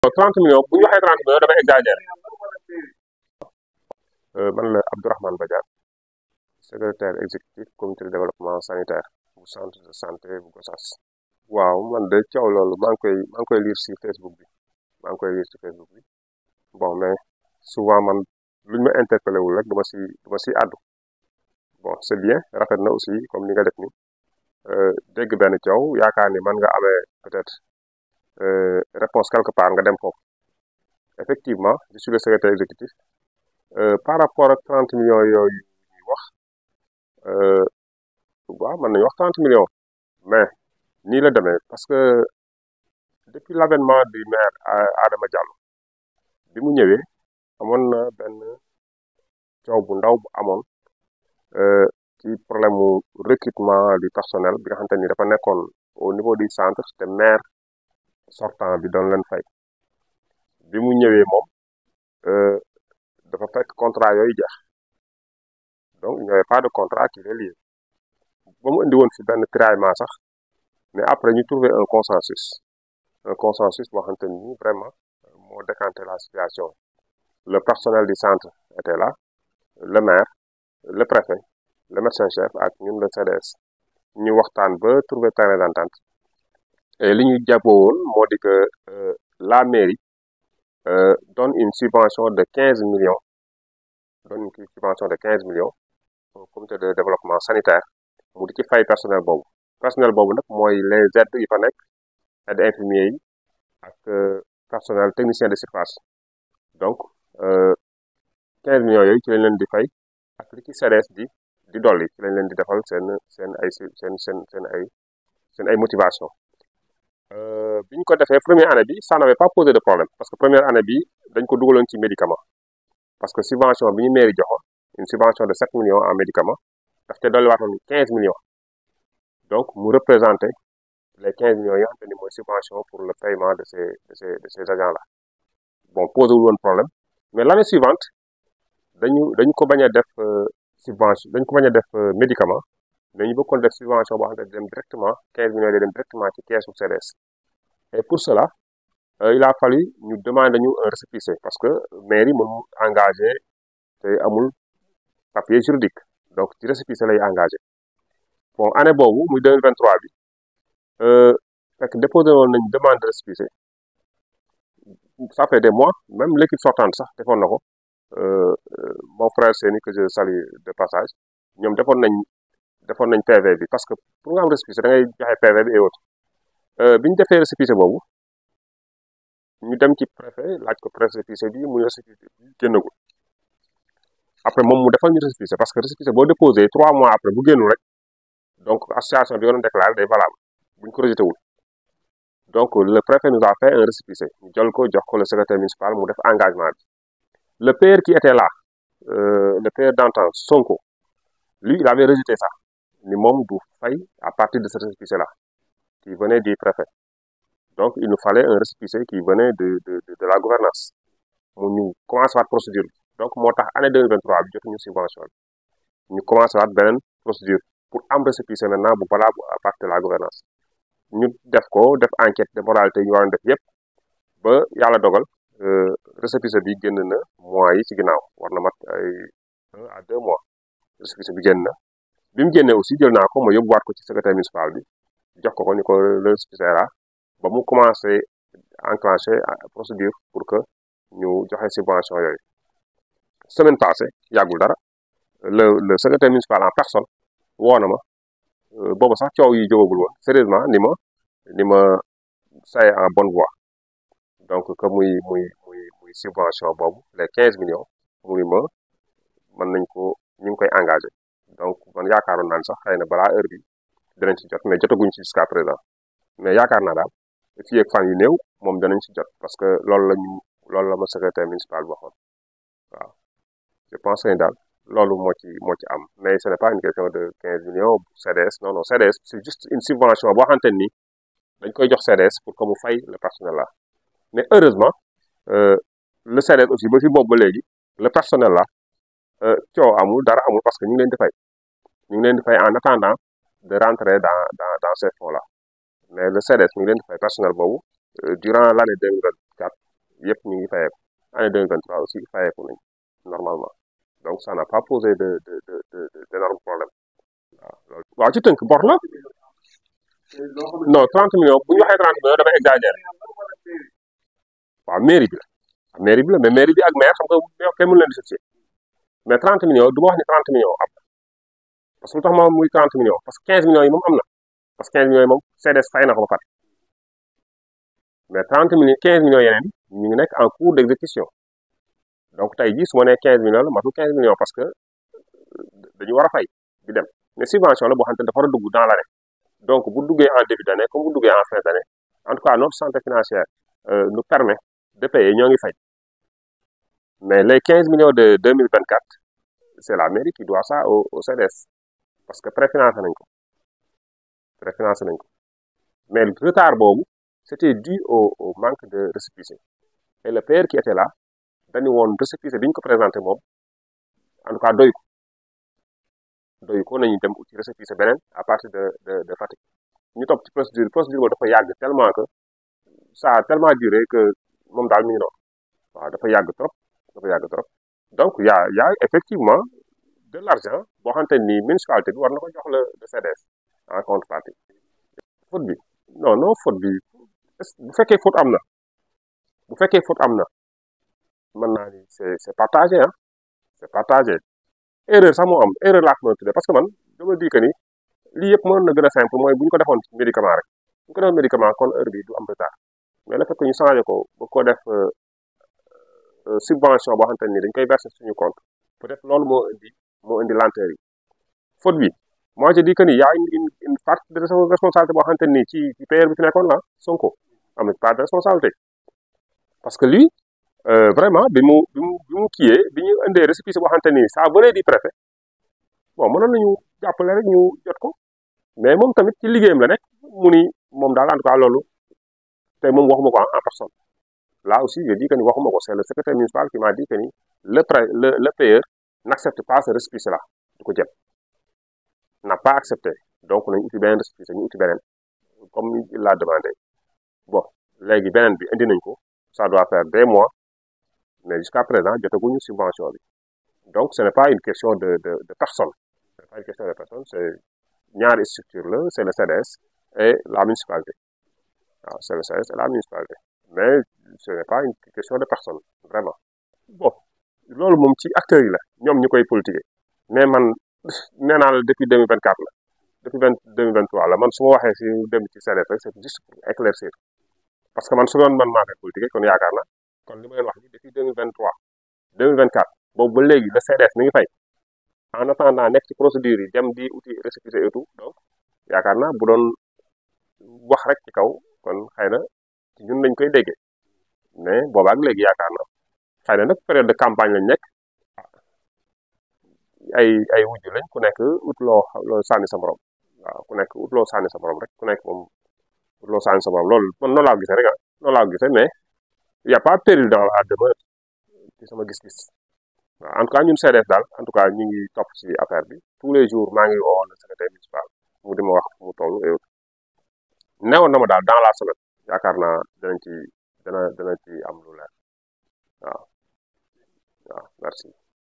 PAPE ABDOURAHMAN BADIANE SECRÉTAIRE EXÉCUTIF C.D.S EXPLIQUE SUR LES 30 MILLIONS .......
PAPE ABDOURAHMAN BADIANE SECRÉTAIRE EXÉCUTIF COMITÉ DE DÉVELOPPEMENT SANITAIRE (C.D.S) GOSSAS DONNE DES EXPLICATIONS SUR LES SOI-DISANTS 30 MILLIONS DE DETTES QUE HONORABLE AMADOU BA TÊTE LISTE PASTEF GOSSAS AURAIT ACCUSÉ LE MAIRE ADAMA DIALLO.
@à la une
Samba Diallo
Boye Ndao
Amadou Ba
Abdourahmane Badiane
ESPRIT
Esprit Gossas
Esprit Sn Tv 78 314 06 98
Moussa Koyate